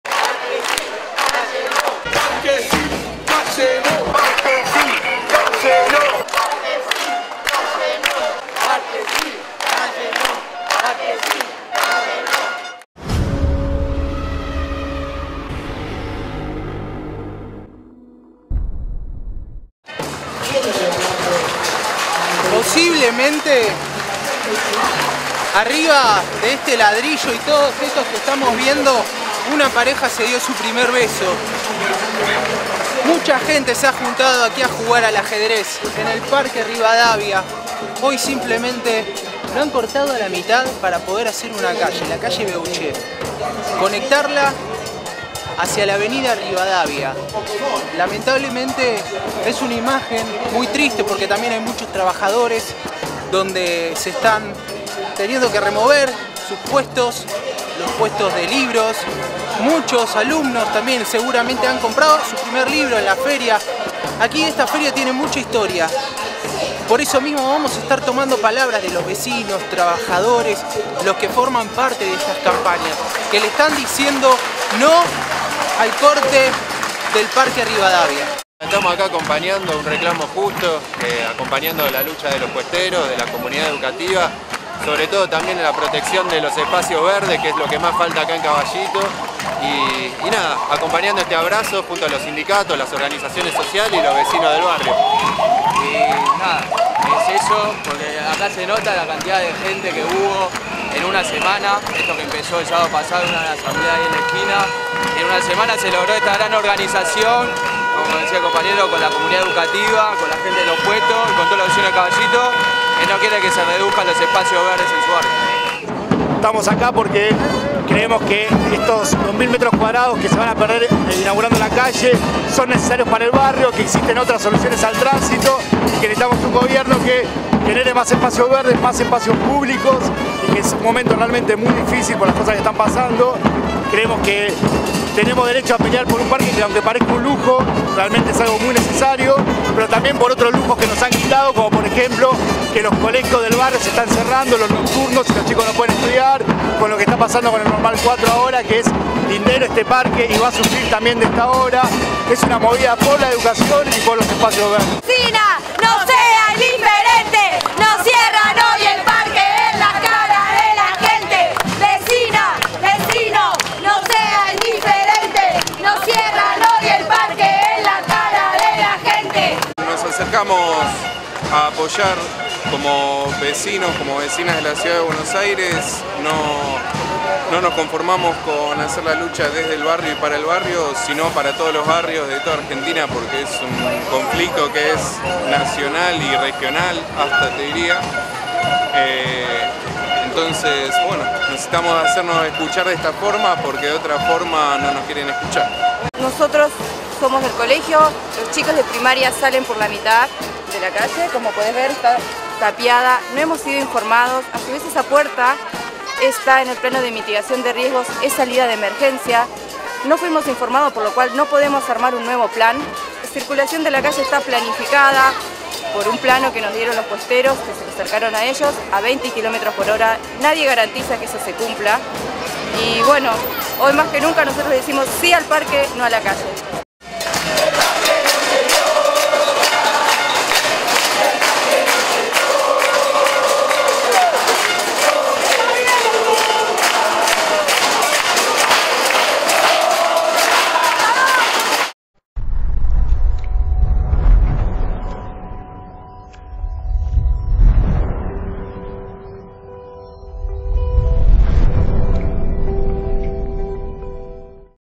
Parque Rivadavia - Vecinos defienden espacio verde
En 1968 se sancionó la Ordenanza 23.475 que reflotó el viejo proyecto de abrirle paso a Beauchef, y en los noventa, se aprobó la Ordenanza 46.229 que prohibía el cambio de destino de todo espacio destinado a parque. Hubo otros intentos, pero en 1996 se aprobó la Constitución de la Ciudad, incorporando los principios de la Ordenanza que establece como principio constitucional que los espacios verdes deben conservarse e incrementarse. A eso se sumó, en 2009, el Plan Urbano Ambiental que tiene jerarquía superior a cualquier ley de la Ciudad y que confirmó que debe mantenerse la integridad de los parques.

Los vecinos pidieron que se declare “la inconstitucionalidad de la parte del Código Urbanístico que autoriza la apertura de la calle Beauchef”, porque “viola el principio de progresividad en materia ambiental establecido en la Ley General del Ambiente y en Tratados Internacionales con Jerarquía Constitucional”.